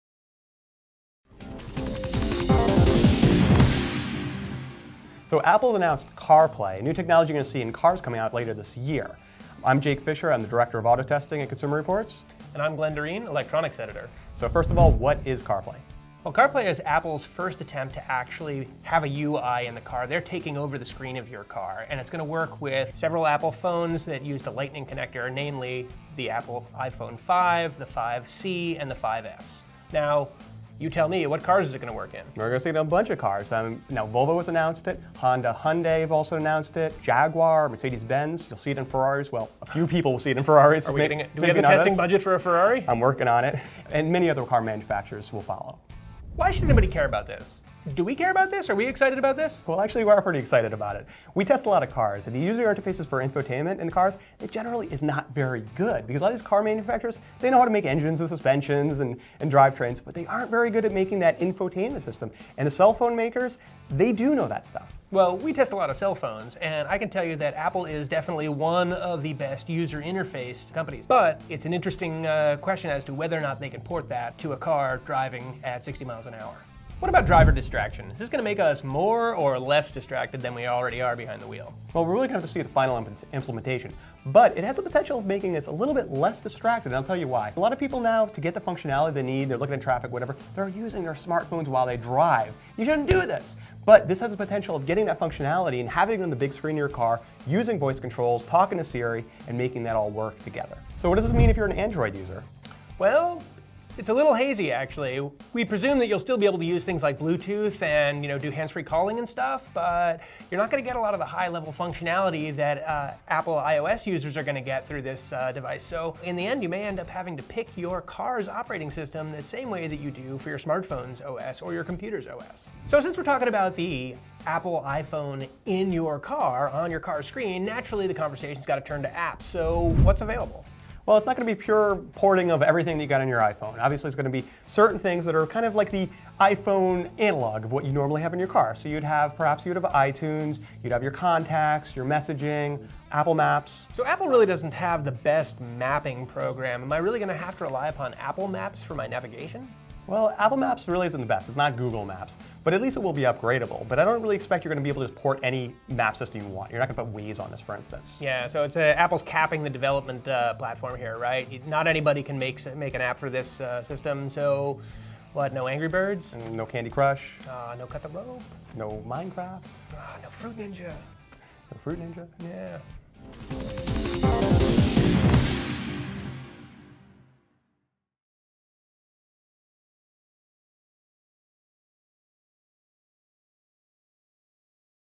Apple CarPlay: iPhone on your dashboard | Consumer Reports
Apple is bringing your iPhone software into your car with its new CarPlay technology. It allows drivers to access apps for maps, music and messaging on a dashboard display.